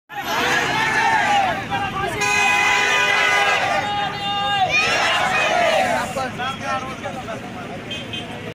साईनपाडा हत्याकांड: भिगवणमध्ये पुणे-सोलापूर महामार्ग रोखला |Prabhat Online News
धुळे जिल्ह्यातील साईनपाडा येथे झालेल्या भिक्षेकरींच्या हत्याकांडाच्या निषेधार्थ भिगवण येथे पुणे-सोलापूर महामार्गावर रास्ता-रोको आंदोलन करण्यात आले.